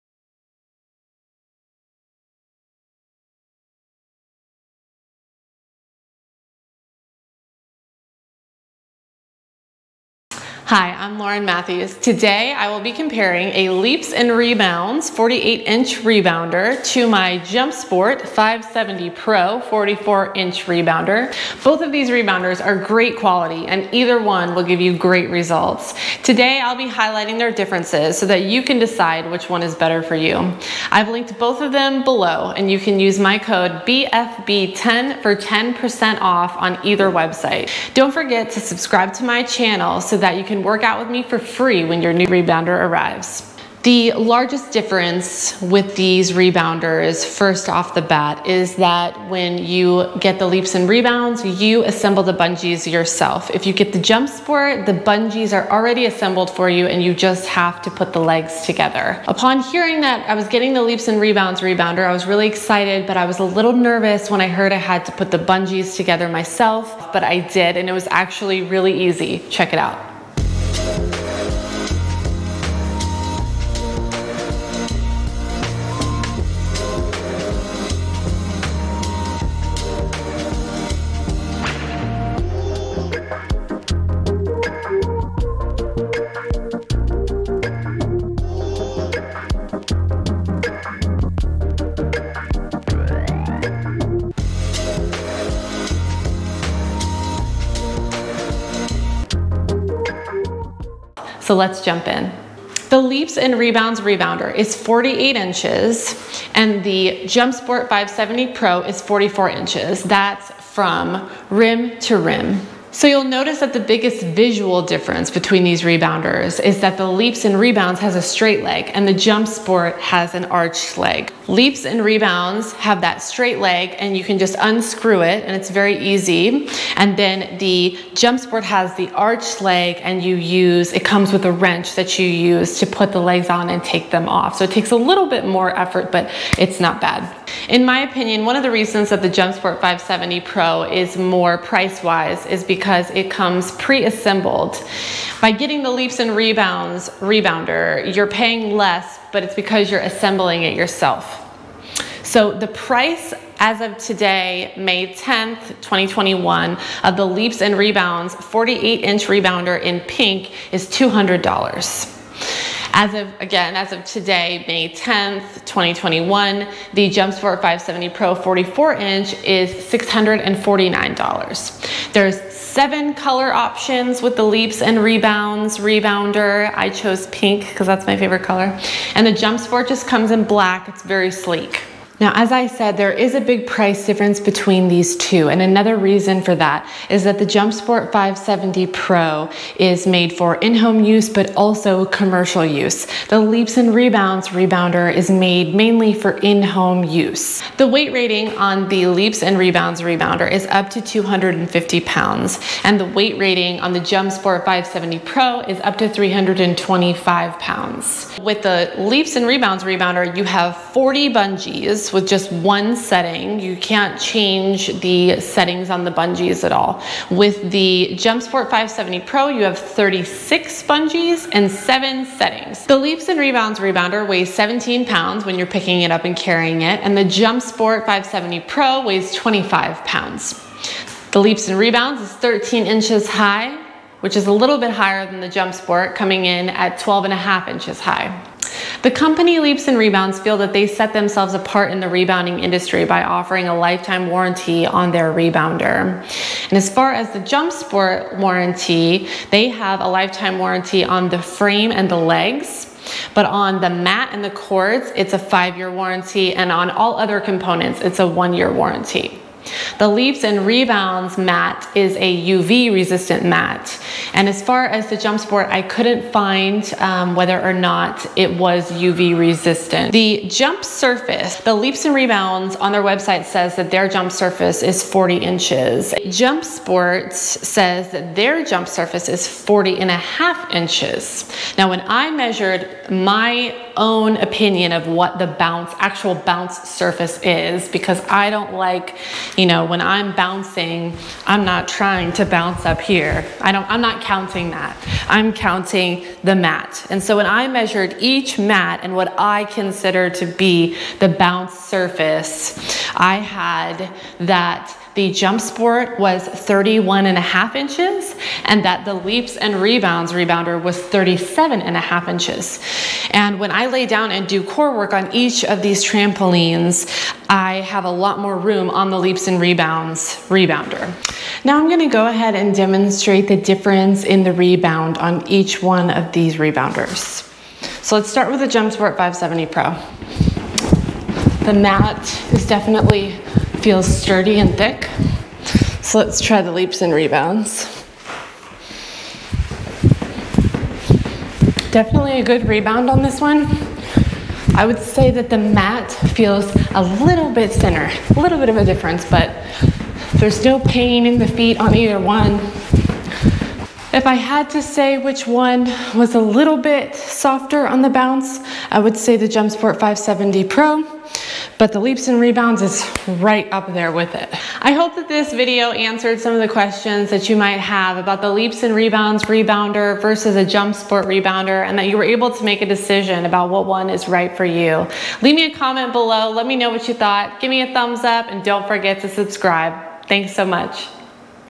JumpSport vs Leaps and Rebounds
The following video contains my personal opinion on two popular rebounders.

Please consult your doctor before beginning any new type of exercise.